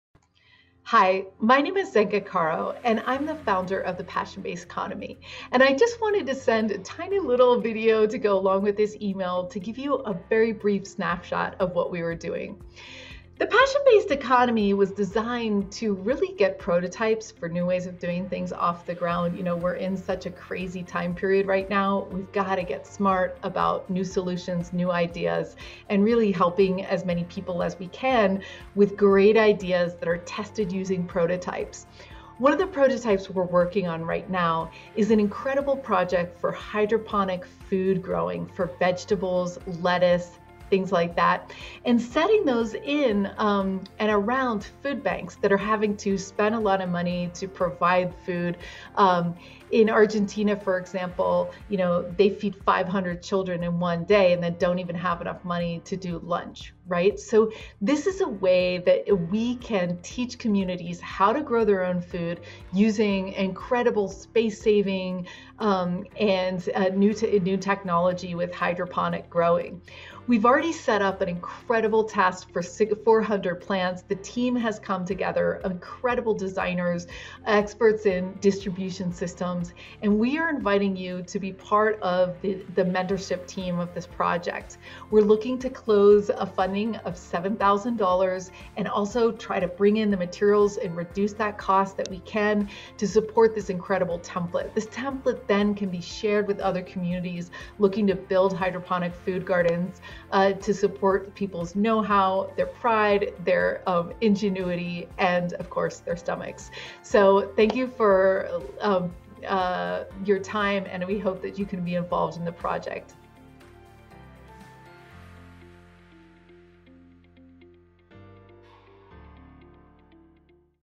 Hydrodome - Hydroponic Organic Food Garden Template - Passion Based Economy
We’re designing a process for people to create their passion projects by gamifying resource procurement. Can we leave behind powerful templates and impact business which can accelerate lifestyle and civilization redesign?